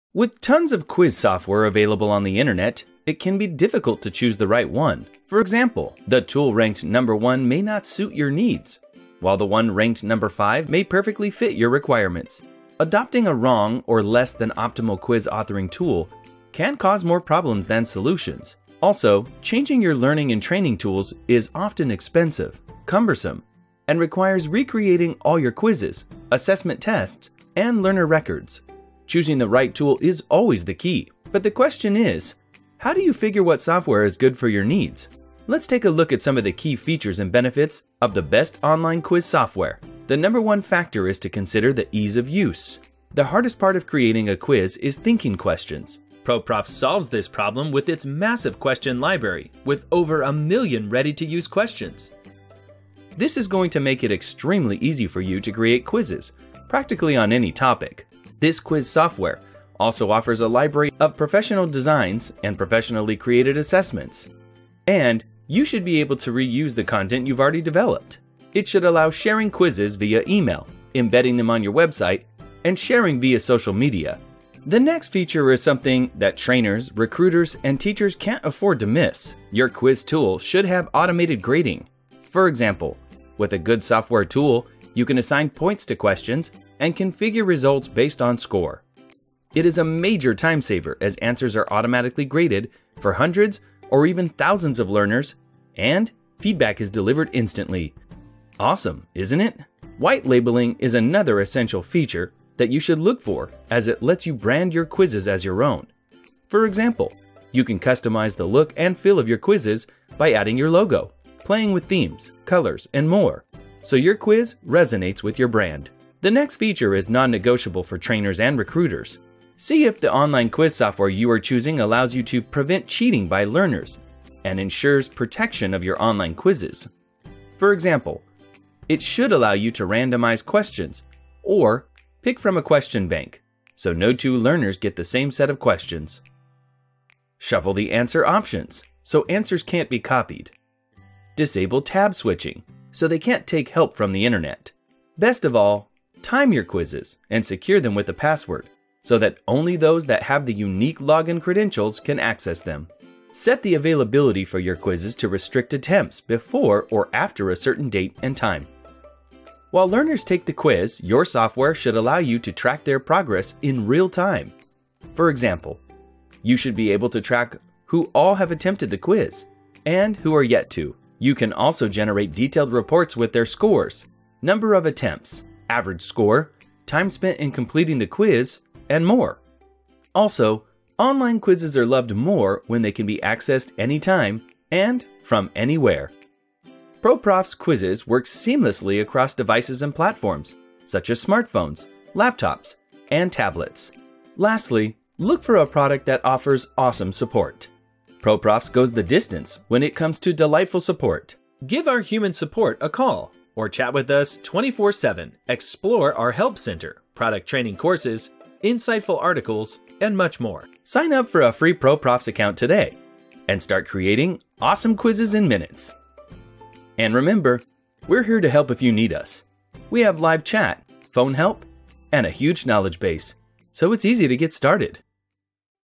How to Choose the Best Quiz Software
How to Choose the Best Online Quiz Software
Choosing the right tool is the key to create the perfect quiz. But the question is - how to choose the best quiz software?

Adopting a wrong or less-than-optimal quiz authoring tool can cause more problems than solutions. Also, changing your learning & training tools is often expensive, cumbersome, and requires recreating all your quizzes, assessment tests, and learner records.

In this video, we’ll walk you through some of the key features and benefits of the best online quiz software and explain why they’re important.

These include:
Ease-of-use
Grading & Feedback
White-labeling
Settings & Configurations
Reports & Analytics
Anytime, anywhere access

Not just the features, the video also talks about the importance of reliable customer support. It could be human support, a call or 24x7 chat support.

Watch the video to know how each of the above-mentioned features contributes to the success of an online quiz software.

To have a better understanding of how the ProProfs Quiz Maker actually works, visit and take a product tour or create your own quiz for free.